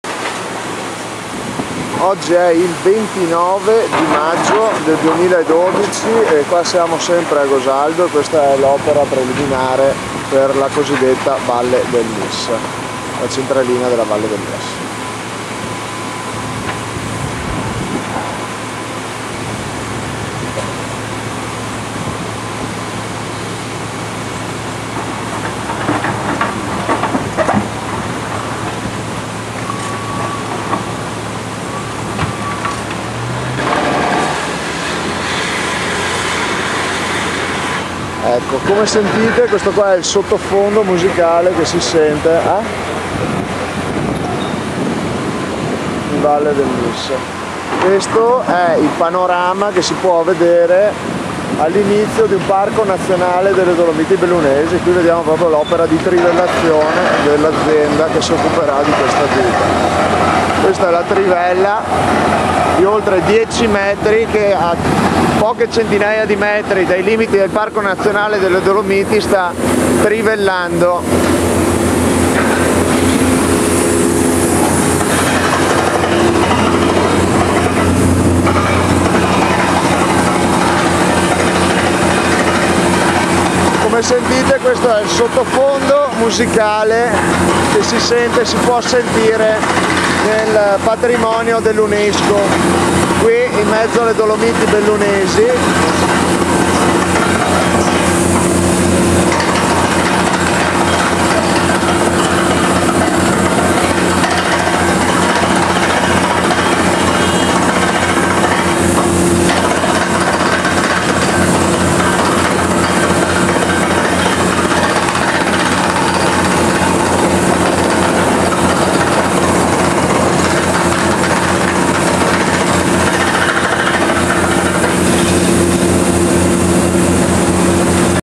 29.05.2012 - Val del Mis - Part. 2
Aggiornamento dei lavori della centralina idroelettrica in Val del Mis